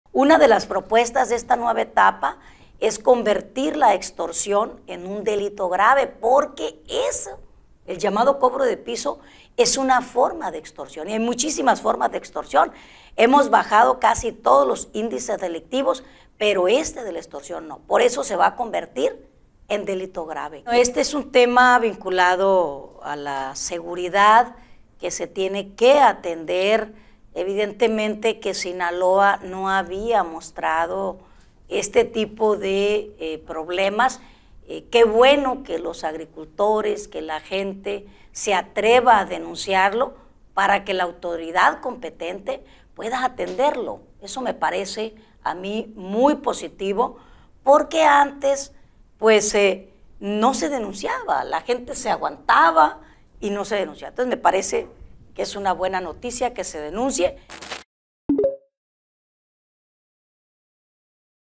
Extorsión será delito grave: Imelda Castro
Tras las denuncias de productores del norte de Sinaloa por “cobro de piso”, la aspirante de Morena al Senado de la República, Imelda Castro Castro, anunció en el Noticiero Noroeste que buscarán que la extorsión sea un delito grave.